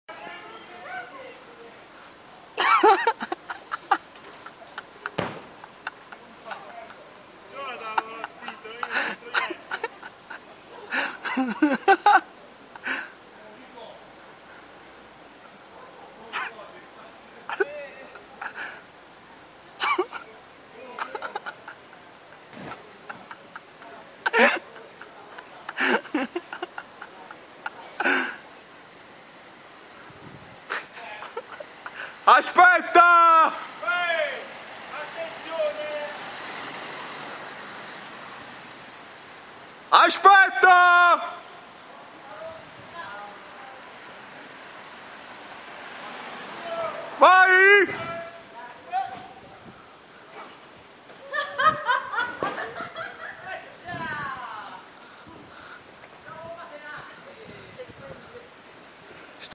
A Casa Madella (Ambasciata Vastese a Pisa) volano anche i materassi dalla finestra
Per far fronte alle invasioni barbariche, gli inquilini di Casa Madella, sono costretti a prendere dei seri provvedimenti arrivando perfino a defenestrare i N.3 materassi infetti, non curanti del pericolo di contagio N.B.C.R.
(Nucleare Batteriologico Chimico e Radioattivo) alle zone limitrofe.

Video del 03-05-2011